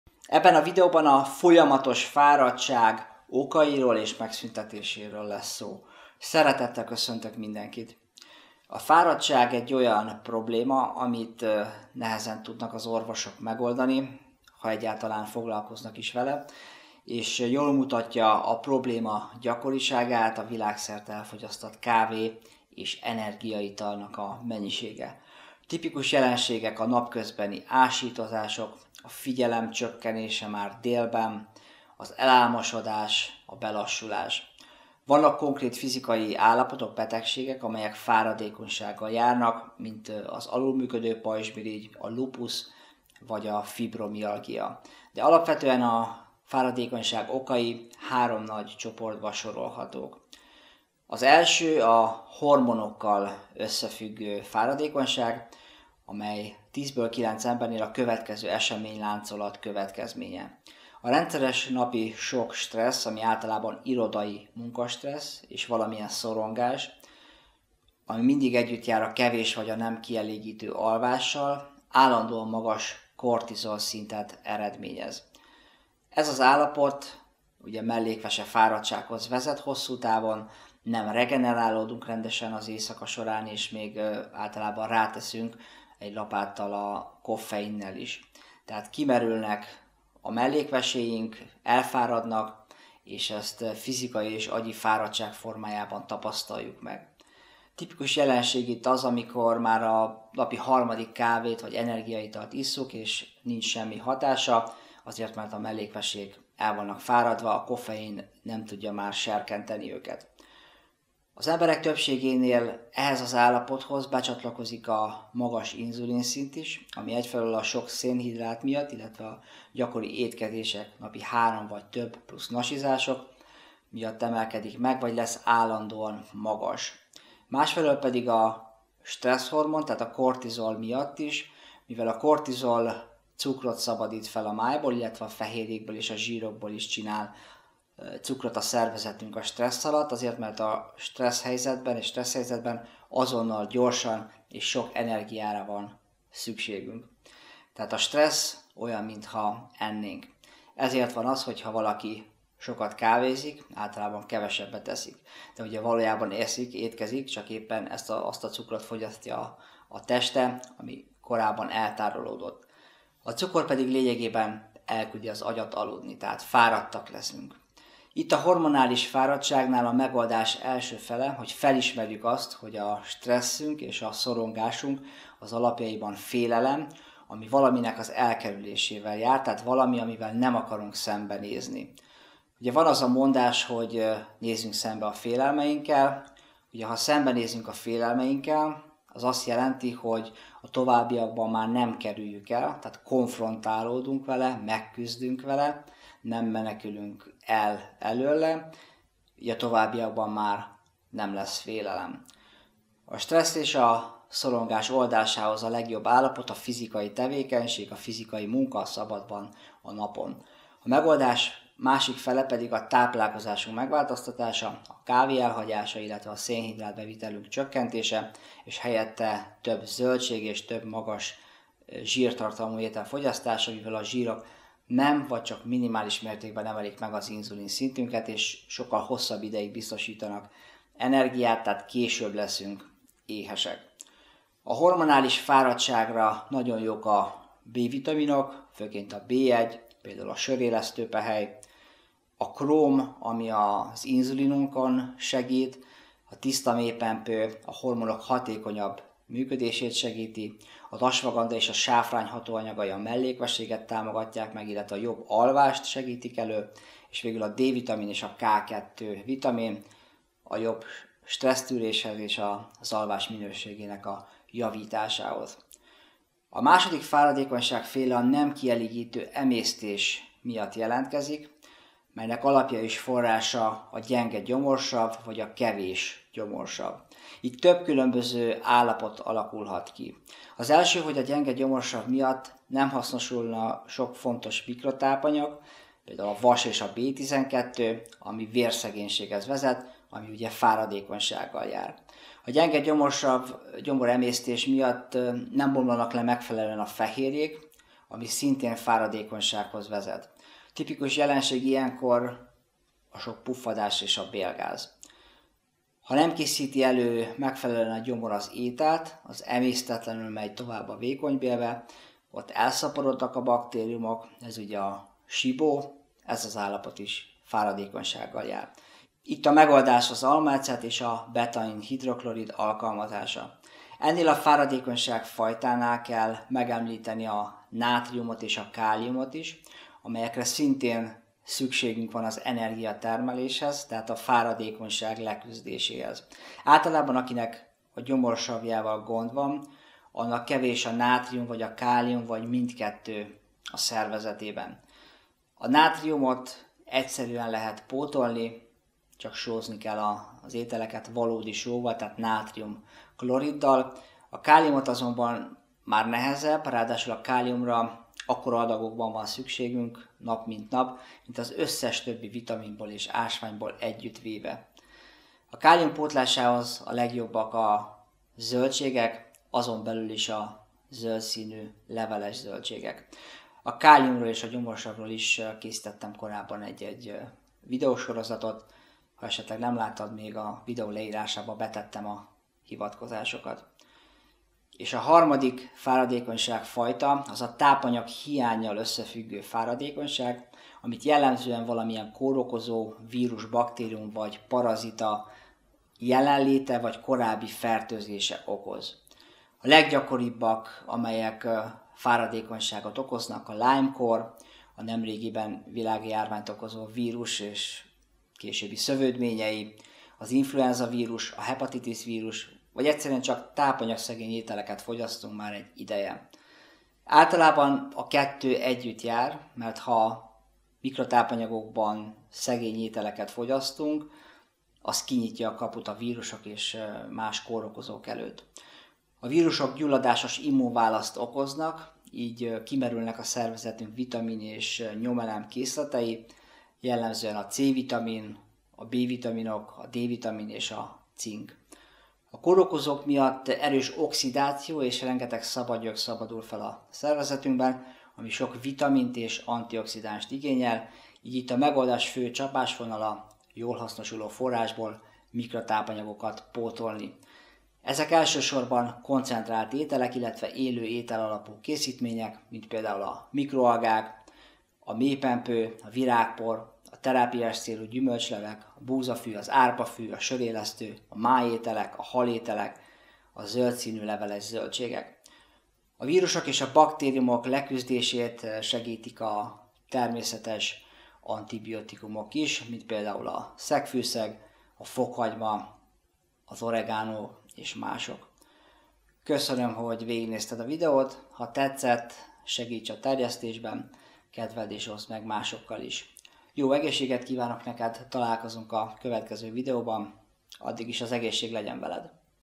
Hogyan szabaduljunk meg a Fáradtságtól?
A fáradtság három nagy okozója. A hormonokkal összefüggő fáradtság, az emésztéssel összefüggő fáradtság és a kórokozók okozta fáradékonyság okai, működése és megoldássuk.